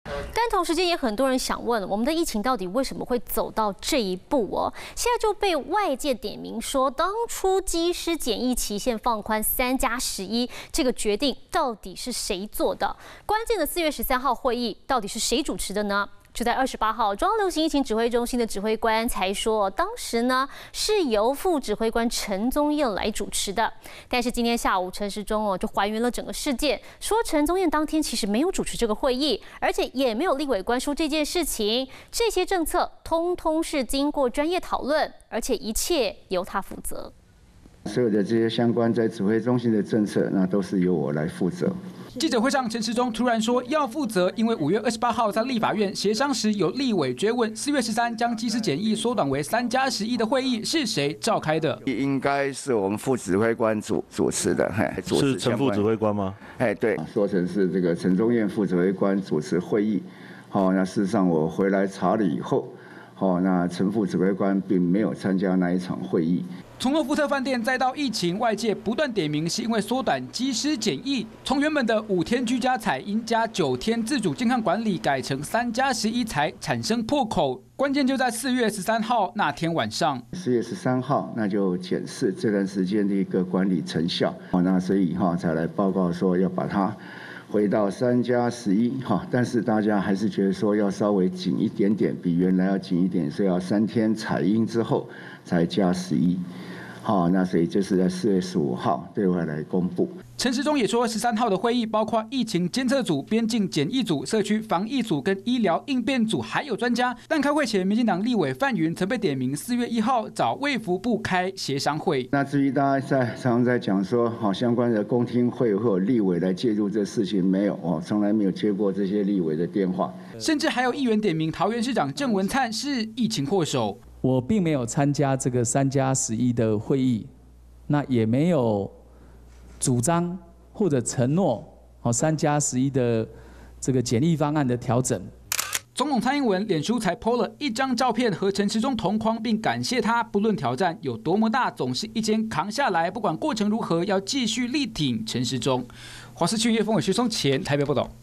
放寬機師檢疫期政策 陳時中:我負責｜華視新聞 20210529
機師檢疫期限放寬「3+11」，不過被外界點名，因這波疫情爆發的主因，關鍵的4月13號會議是誰主持，28號中央流行疫情指揮中心指揮官才說，當時是由副指揮官陳宗彥主持，不過今天下午，陳時中還原整起事件，說陳宗彥當天並沒有主持會議，也沒有立委關說，而且這些政策，也都經過專業討論，一切都由他負責。